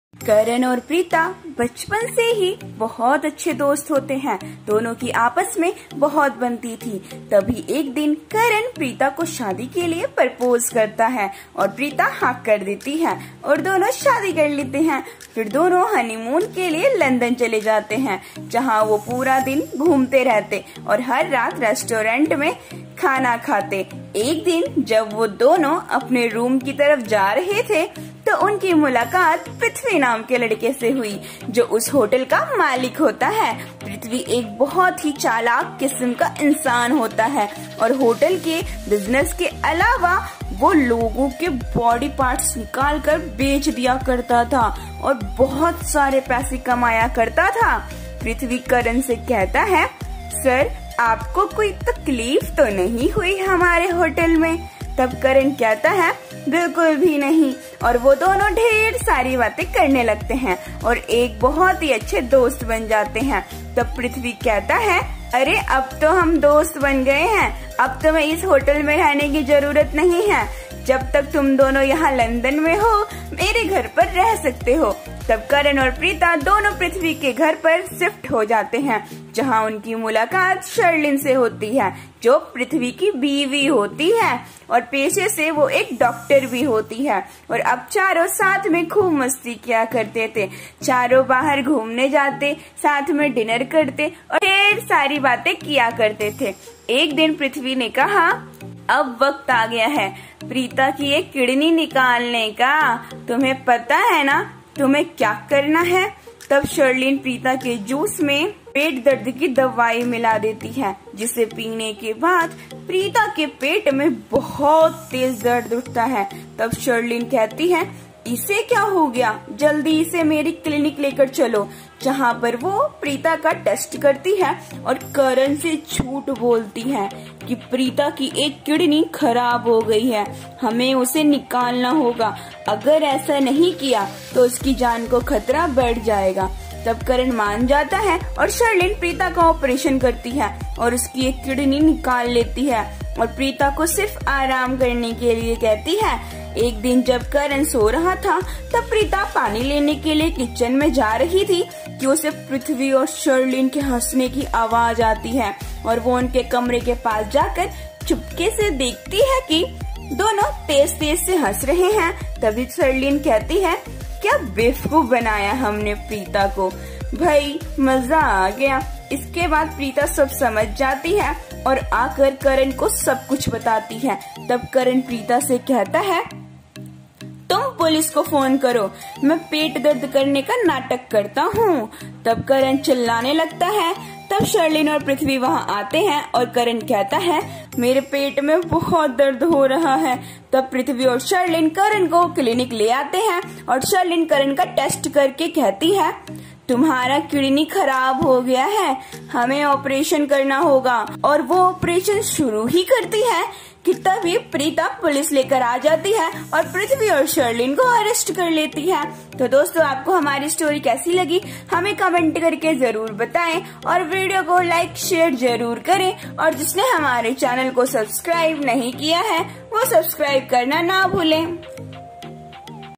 धोखेबाज दोस्त ! Kundali bhagya ! Karan Luthra ! Preeta ! Mahira ! Prithvi ! Rishabh Luthra !
Topic-Cheater Friend

 Serial Kundali Bhagya

 * Karan * and * Preeta * are very good friends since childhood, both of them used to make a lot of each other, then one day * Karan* proposes to * Preeta * for marriage and * Preeta * does it and both get married.  Then both of them go to London for honeymoon where they roam all day and eat food in restaurant every night, one day when both of them were going towards their room, they met a boy named Prithvi who was with him.  The owner of the hotel * Prithvi * is a very clever person and apart from the business of the hotel, he used to sell the body parts of the people and earn a lot of money * Prithvi * says to *Karan*  Sir, you did not have any problem in our hotel then *Karan* says not at all and they both start talking a lot and become a very good friend then *Earth* says hey now we have become friends  Now you don't need to stay in this hotel as long as you both are here in London you can stay at my house then *Karan" and *Preeta* both shift to *Prithvi*'s house where they meet *Sherlyn*  would have  Who is the wife of *Earth* and she is a doctor by profession and now all four used to have a lot of fun together, all four used to go out to have dinner together and talk a lot together one day *Earth*  Where is the time to remove one kidney of * Preeta * you don't know what you have to do, then * Sherlyn* mixes stomach pain medicine in the juice of * Preeta * after drinking which * Preeta * has a lot in her stomach.  Severe pain arises then *Sherlyn* says what has happened to it soon take it to my clinic where she tests *Preeta* and lies to *Karan* that *Preeta* has one kidney damaged by us.  He will have to be removed, if he does not do this, then his life will be in danger, then * Karan * agrees and * Sherlyn * operates * Preeta * and takes out one of her kidneys and tells * Preeta * just to rest  One day when *Karan* was sleeping then *Preeta* was going to the kitchen to get water that she hears *Prithvi* and *Sherlyn* laughing and she goes near their room and watches secretly.  that both are laughing loudly then  Shri *Sherlyn* says what a fool we made *Preeta* have fun after that *Preeta* understands everything and comes and tells everything to *Karan* then *Karan* tells *Preeta* you to the police  Call me I pretend to have stomach ache and *Karan* starts screaming then *Sherlyn* and *Prithvi* come there and *Karan* says my stomach is hurting a lot then *Prithvi* and *Sherlyn*  * Karan brings * to the clinic and tests * Sherlyn * * Karan* and says that your kidney is damaged, we have to do the operation and she starts the operation that only then * Preeta * brings the police and *  Prithvi* and arrests Sherlyn.

 So friends, how did you like our *story*, do tell us by commenting * and * like * * share * the video and whoever has not * subscribed * to our channel, do not forget to * subscribe *.